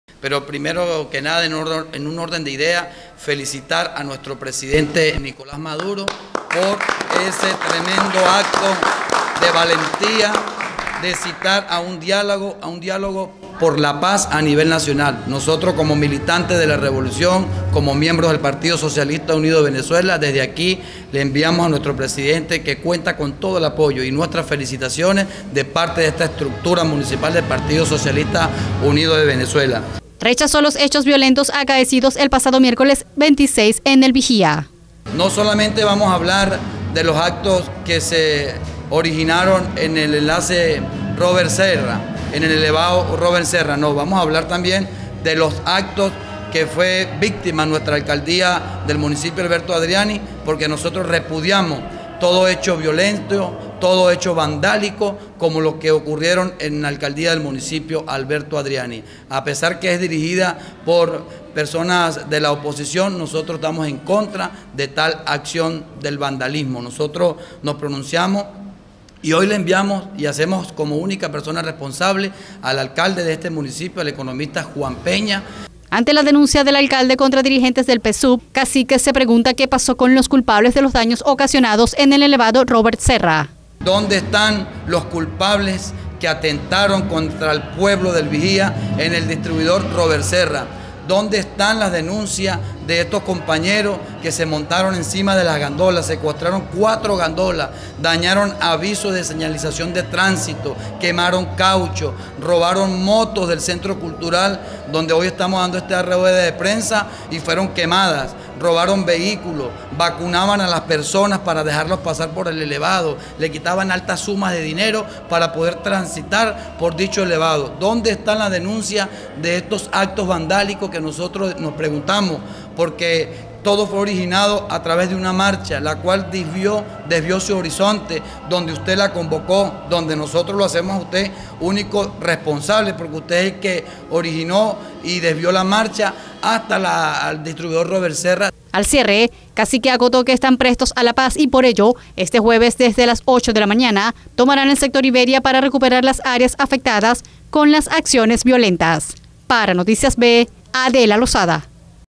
PSUV REPUDIARON LOS HECHOS VIOLENTOS
REPRESENTANTES DEL PSUV ENCABEZADOS POR GERSON CACIQUE, REPUDIARON LOS  HECHOS VIOLENTOS ACAECIDOS EL PASADO MIÉRCOLES EN EL VIGÍA Y EXIGEN AL ALCALDE DENUNCIAR A LOS RESPONSABLES DE LAS ACCIONES EN EL ELEVADO ROBERT SERRA